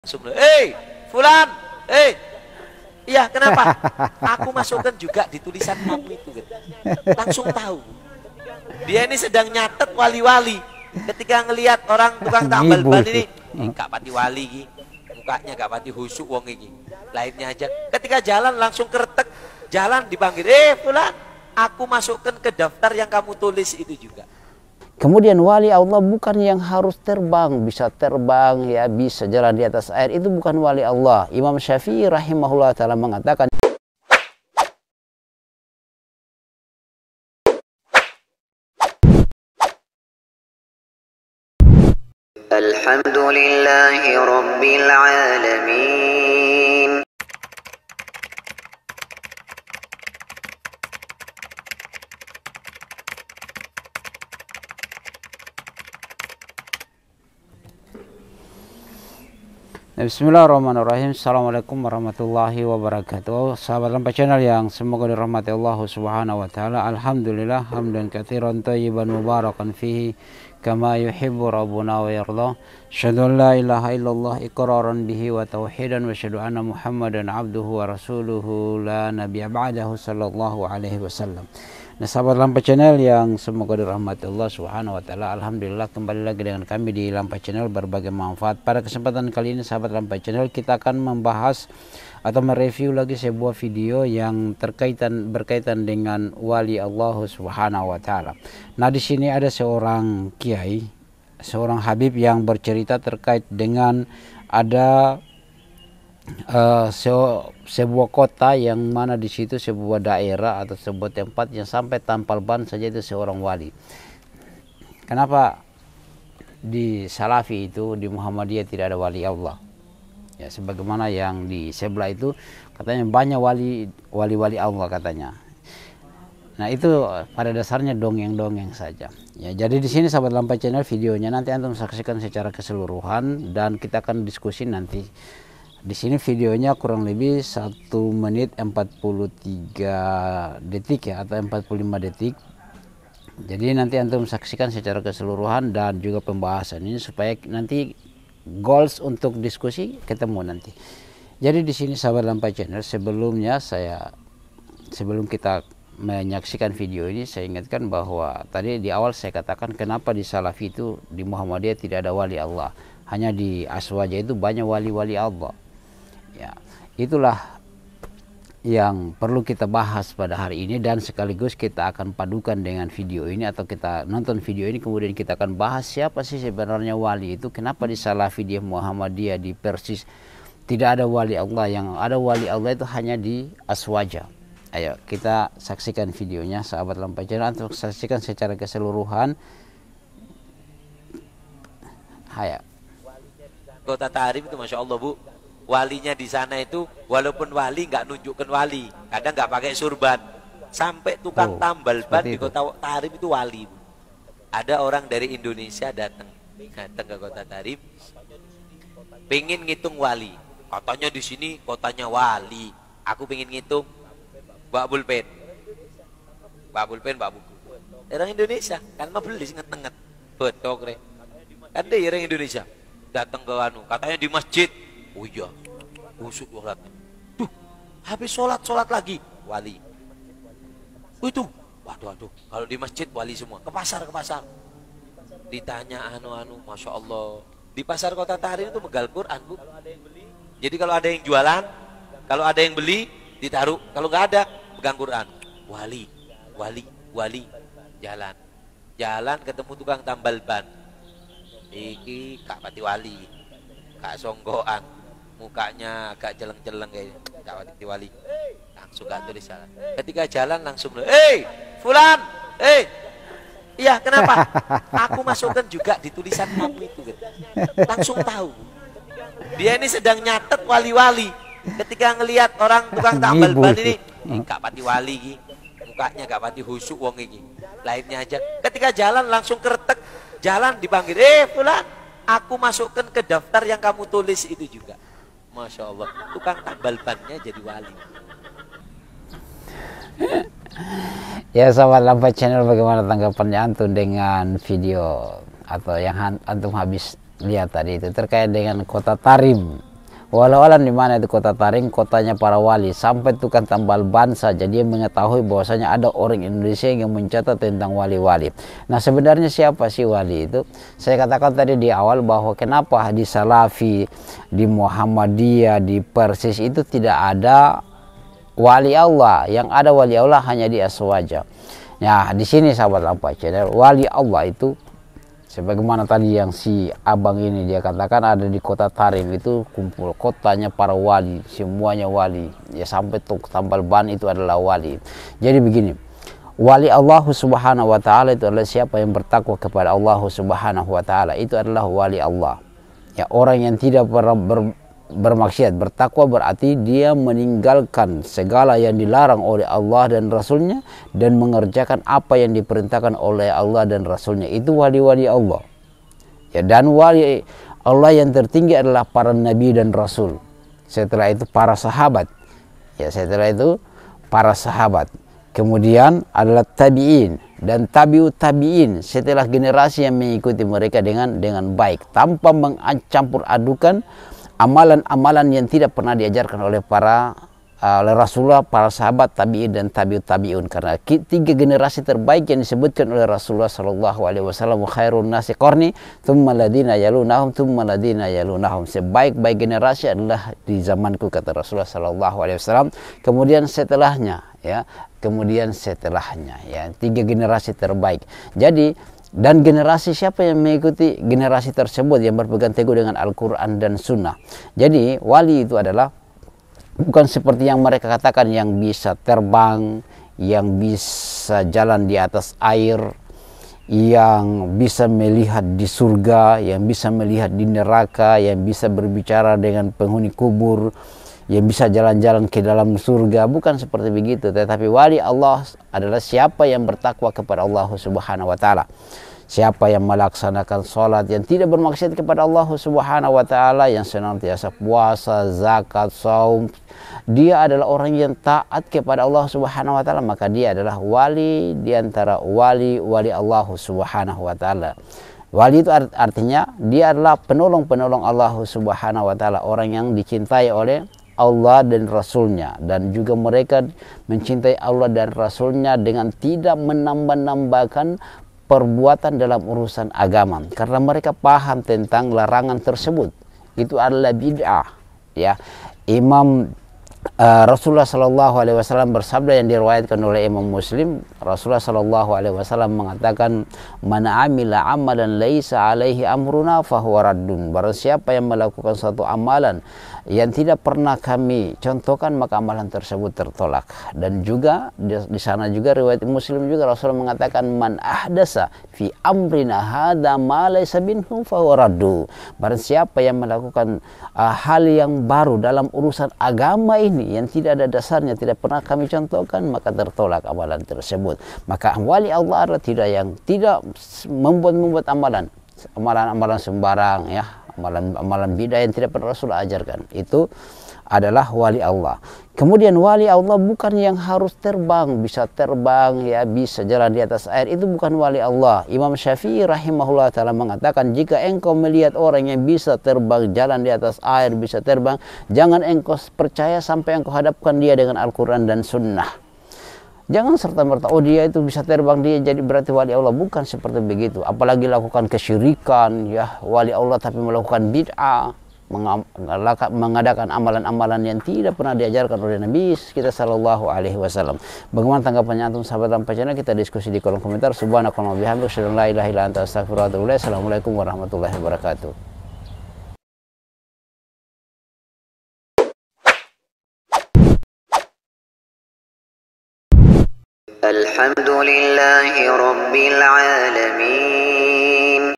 Kenapa Wali Allah Hanya Di ASWAJA Tapi Salafy, Muhammadiyah dan Persis Tidak Ada
Dukung terus dakwah lampai Channel berbagi manfaat bersama Lampai Channel dalam meraih amal jariah sebanyak-banyaknya untuk bekal di hari kemudian